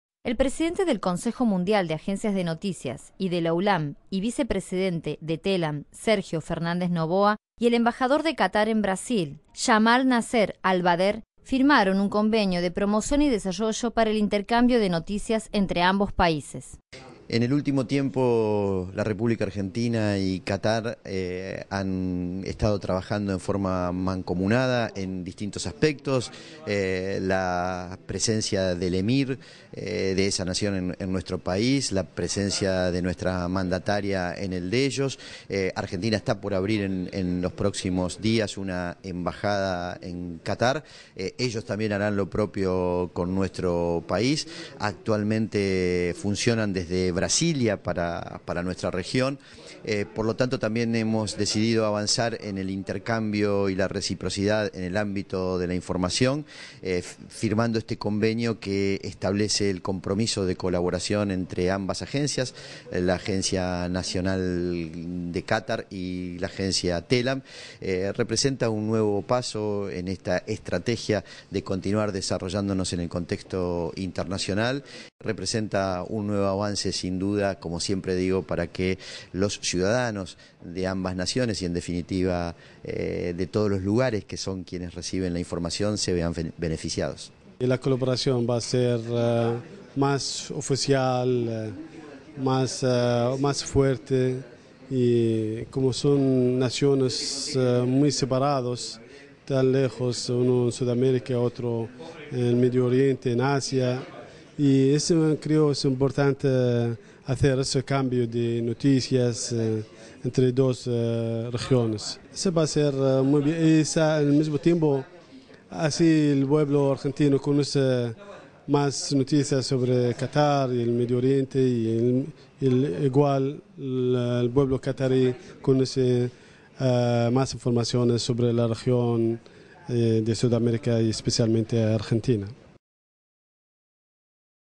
Télam firma convenio con la agencia nacional de noticias qatarí
La agencia nacionalde noticias Télam y la UNA (agencia nacional de noticias del
Emirato de Qatar) firmaron hoy un convenio de intercambio de acceso
a los productos informativos.
"Argentina y Qatar trabajan en forma mancomunada, el Emir
visitó nuestro país, nuestra mandataria los visitó a ellos, y se
están abriendo embajadas en ambos países. Hemos decidido avanzar en
el intercambio y reciprocidad de información", afirmó el
vicepresidente de Télam, Sergio Fernández Novoa.
En ese sentido, consideró que es un "nuevo paso para que los
ciudadanos se vean beneficiados" por el intercambio.
Por su parte, el embajador de Qatar en Brasil (desde donde el
emirato dirige las relaciones diplomáticas con la región hasta en
tanto se efectivice la apertura de su embajada en Buenos Aires),
Jamal Nasser Al-Bader, dijo a Télam que el convenio representa "un
adelanto en las relaciones" entre ambas naciones.
"El convenio permitirá una cooperación más oficial y fuerte
entre los dos países. Como somos naciones muy lejanas, es
importante el intercambio de noticias. El pueblo qatarí podrá
conocer más sobre la región", consideró.
El convenio rubricado contempla compartir textos, fotos y
producciones audiovisuales realizadas por ambas agencias, e incluye
una cláusula para la capacitación de periodistas.